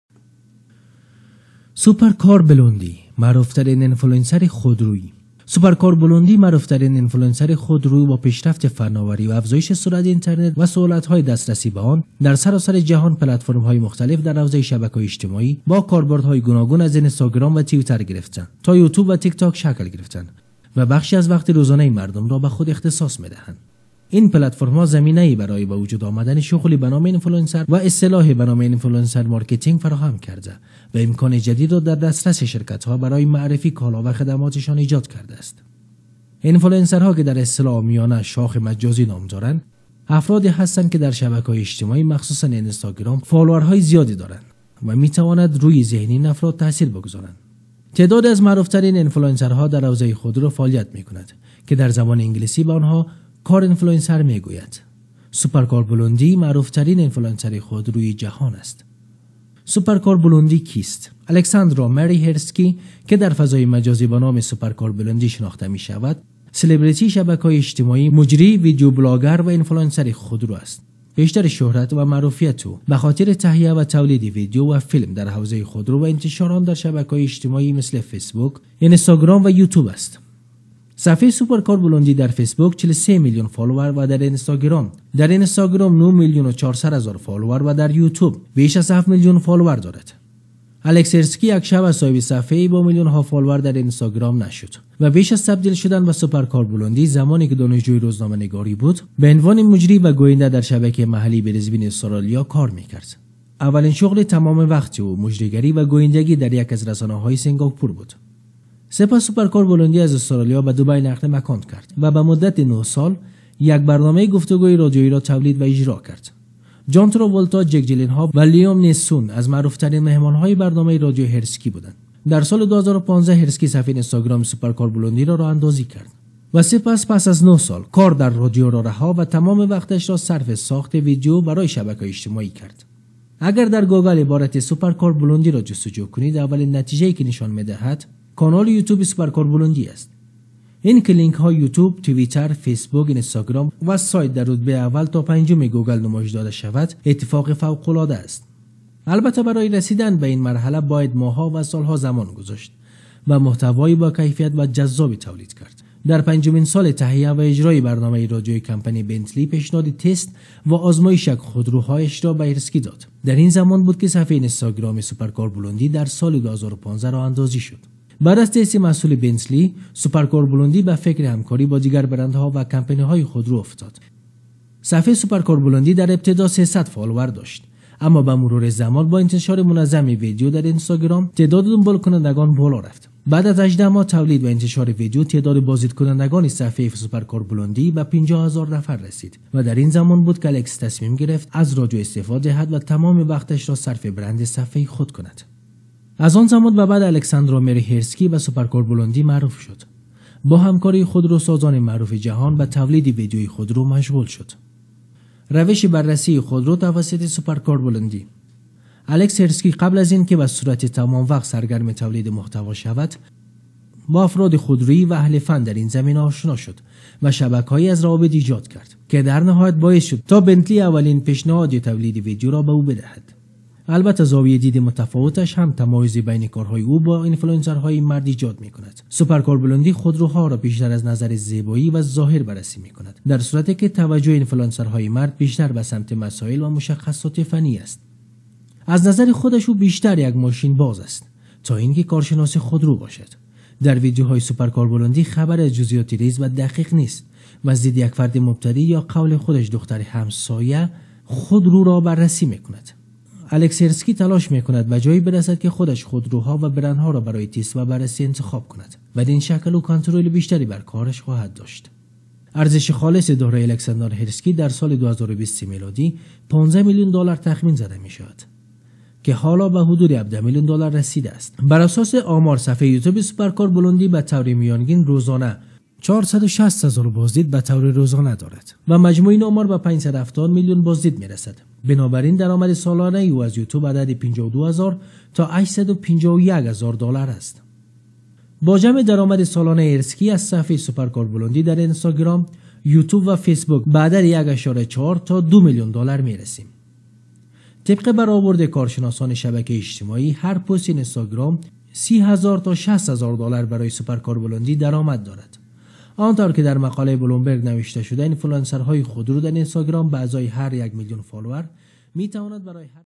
سوپر کار بلوندی کیست؟ Who is supercar blondi?
سوپر کار بلوندی کیست؟
Who is supercar blondi?
در این ویدیو بر خلاف ویدیو های دیگر ما در مورد یکی از موفقترین انفلوئنسر شبکه های اجتماعی پرداخته ایم.  که تولید ویدیو و محتوا در مورد خودرو ها به یکی از موفقترین افراد مبدل شده است.